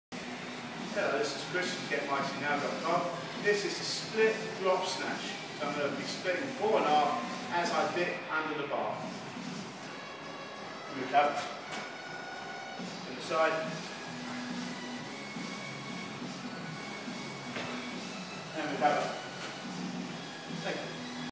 Oly Lift Coaching
Chris shows Split Drop Snatch